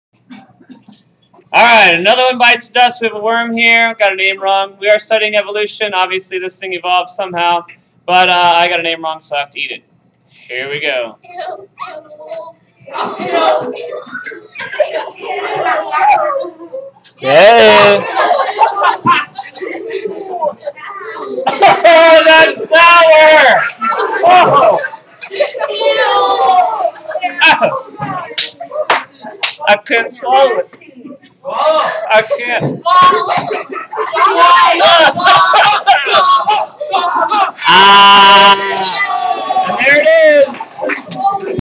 Mr. Wilson eats a Worm.
Worms evolved to has a specific set of adaptations. Let's see if they can survive this new environment.